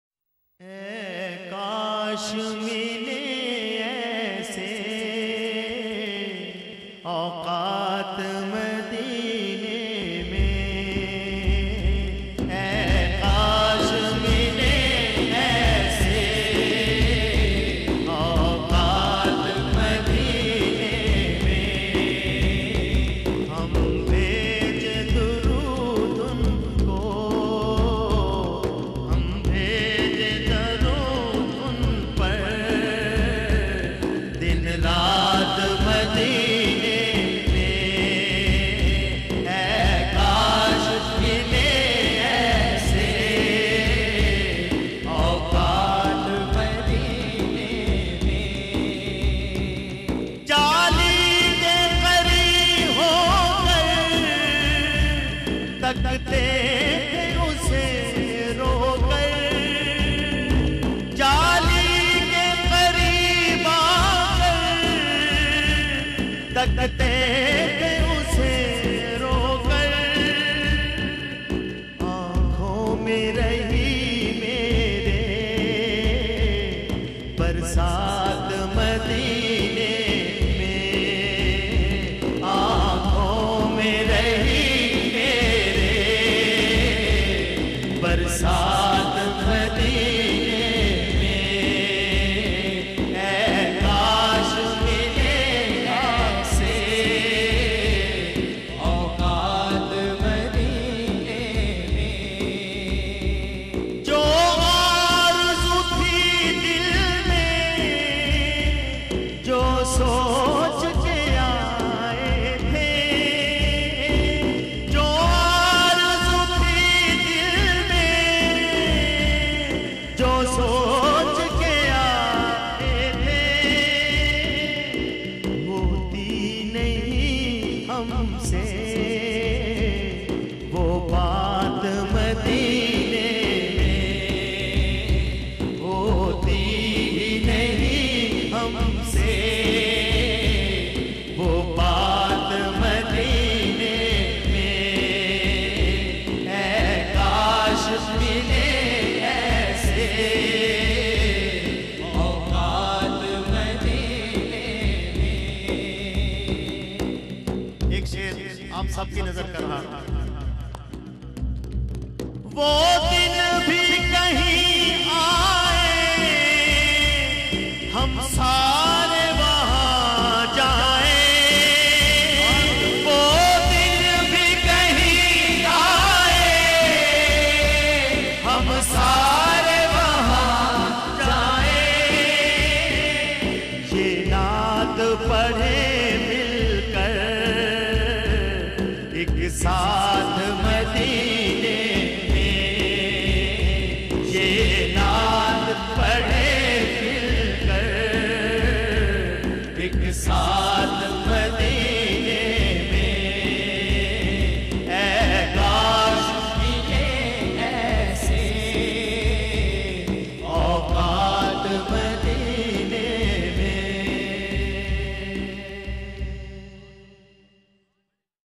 Noor ka Saman G-10/3, Professor Abdul Rauf Rufi -8
Mehfil e Naat 26th March at G-10/3, Islamabad Upload By Maqsood Ahmad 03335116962